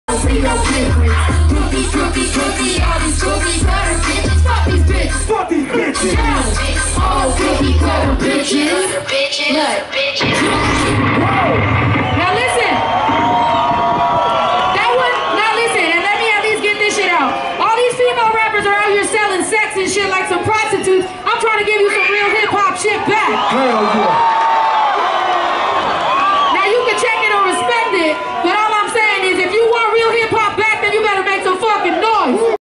Snow Tha Product - Live in El Paso
Snow Tha Product performs live at the Texas Showdown Fest in El Paso, TX on Sunday, July 20, 2014, at the County Coliseum.